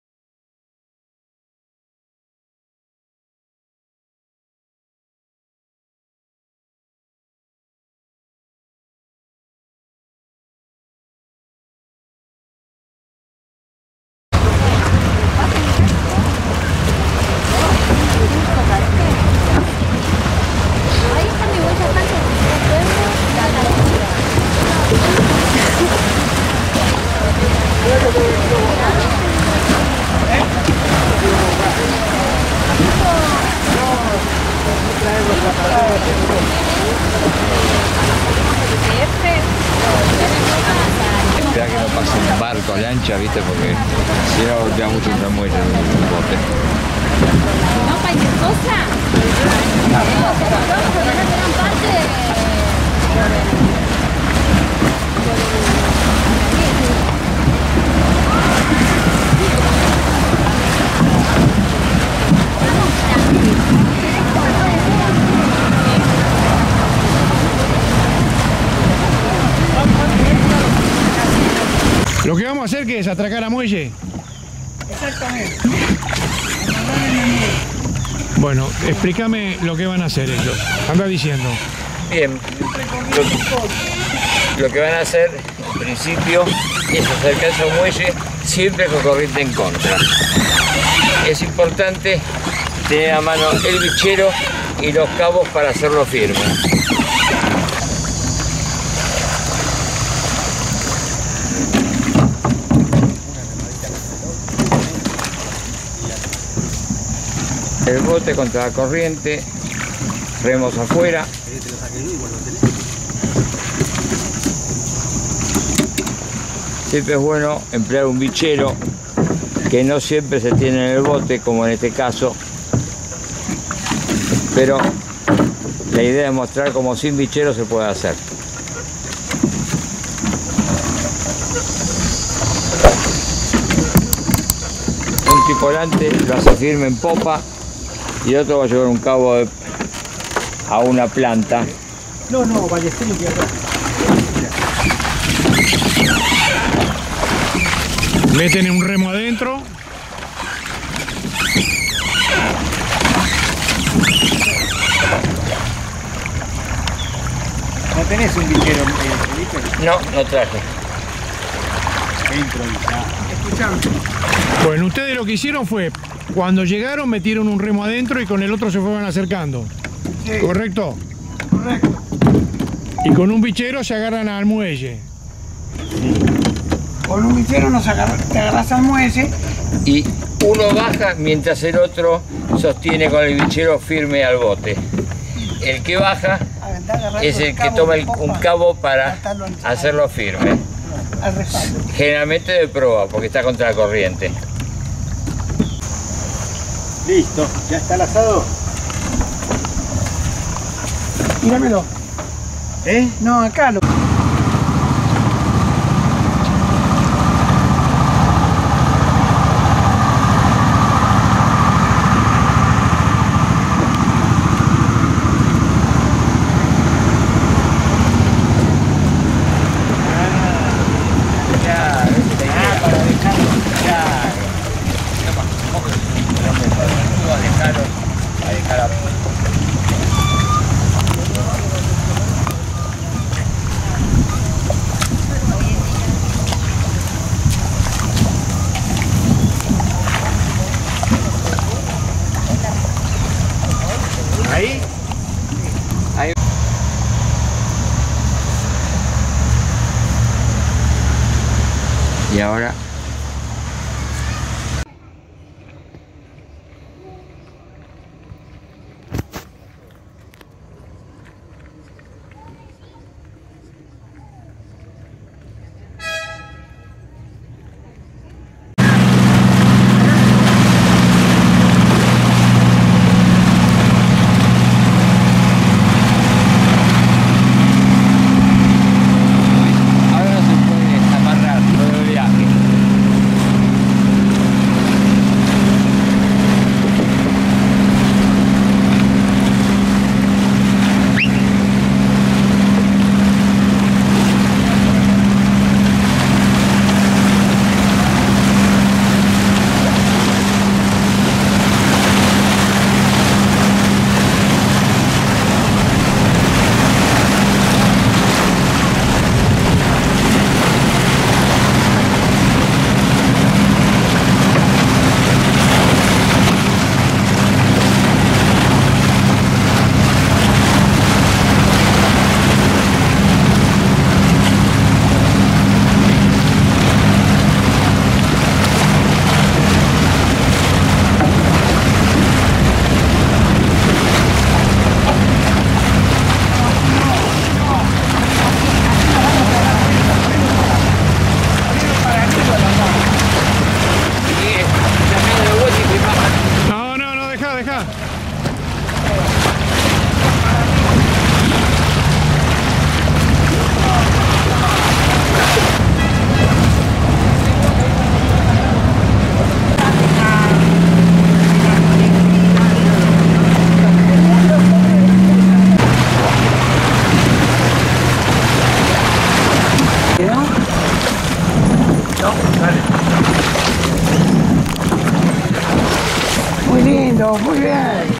Bote de remos . Atracar y desatracar, Embarcar y desembarcar
Queremos mostrar las formas seguras  para los botes de remos en lo que es la forma de atracar y desatracar y la de embarcar y desembarcar